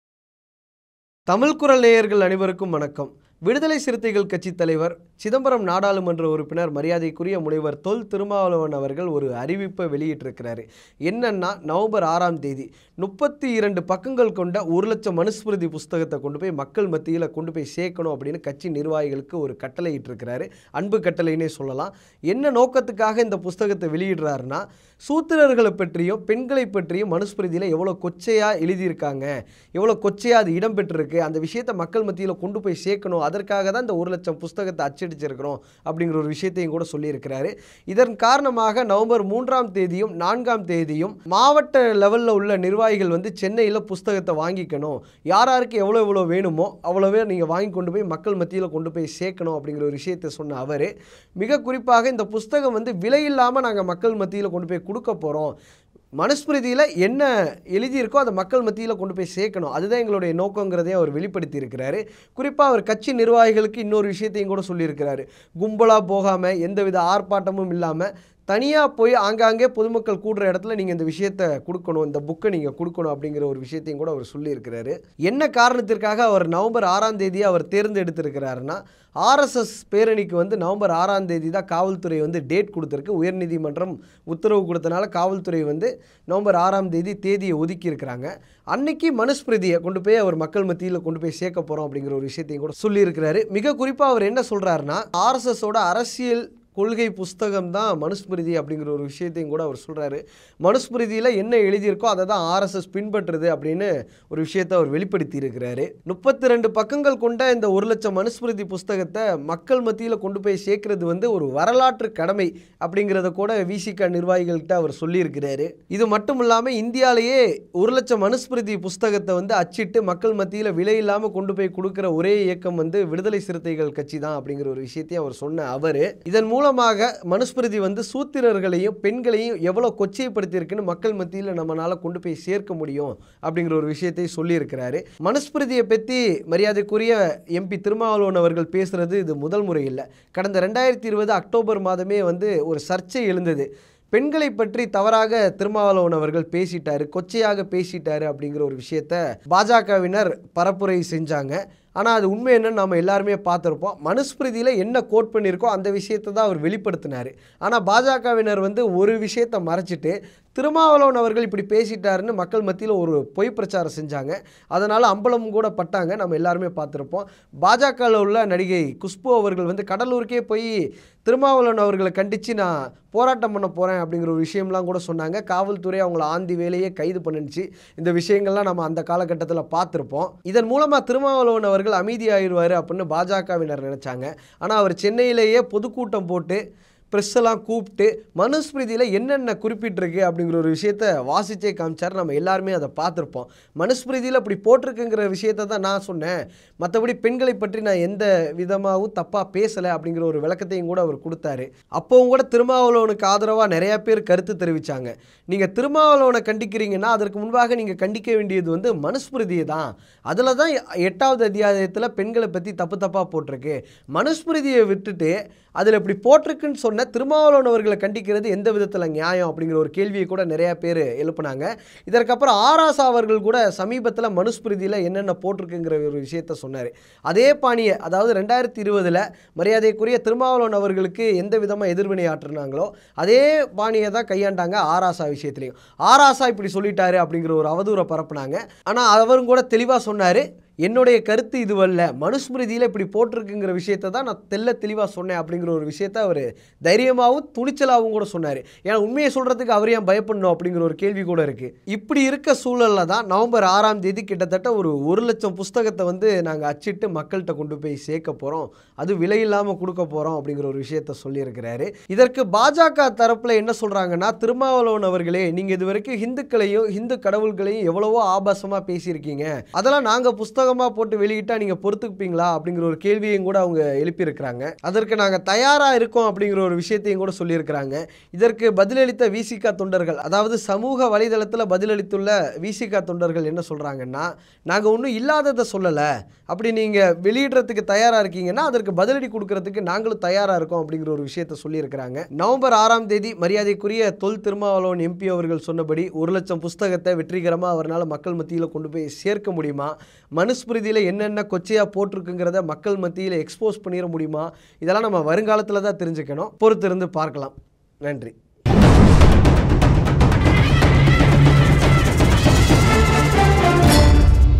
சங்கிகளுக்கு திருமா வைத்த ஆப்பு மனுஸ்மிருதியை அம்பலப்படுத்தும் விசிக |Tamil Kural
VCK President Thol Thirumavalavan mp has said that one lakh Manusmriti books will be distributed to the people. He also said that he is doing this as a way to highlight to the people how portrayed the Shudras and women are in the Manusmriti.